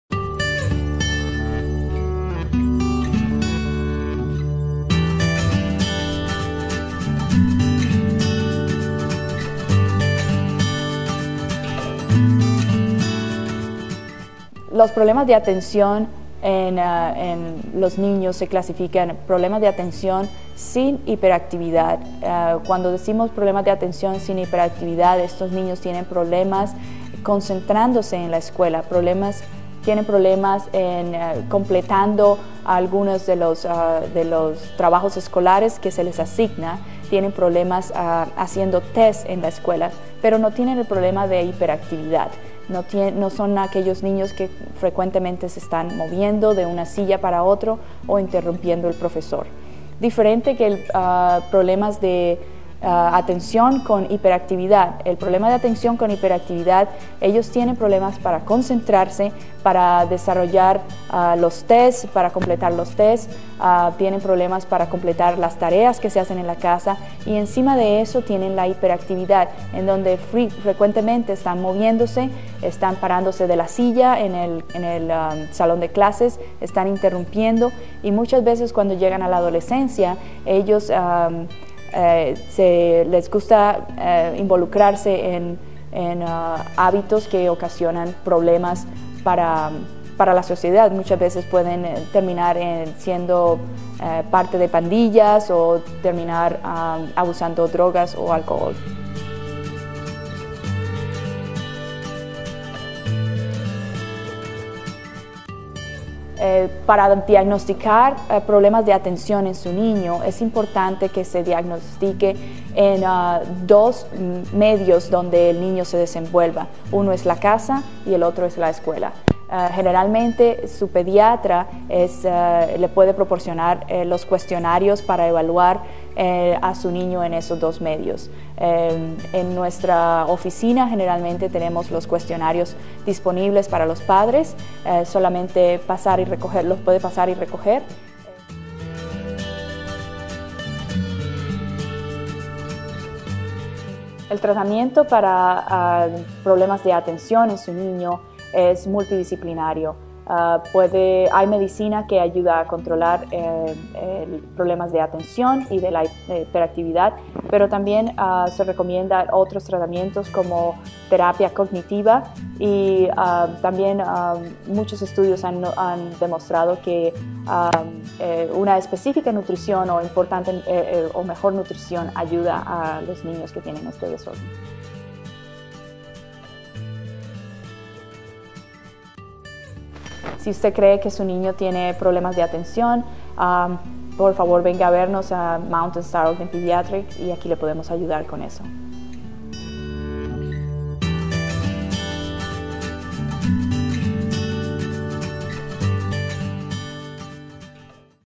Trastornos de atención en los niños
En este video el Dr. Isabel Christina Lau describe los síntomas de los trastornos de atención en los niños, y analiza cómo se diagnostican y tratan estos trastornos.